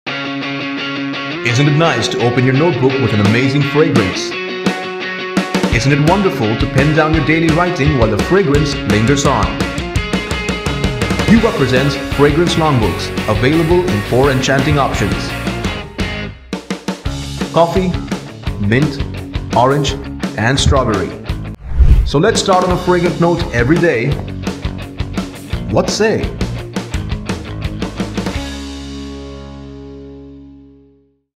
Introducing Youva Fragrance Longbooks Collection!
Immerse your senses with our new Fragrance notebooks with enticing covers. Explore the captivating essences of coffee, orange, mint, and strawberry. Which fragrance captivates you the most?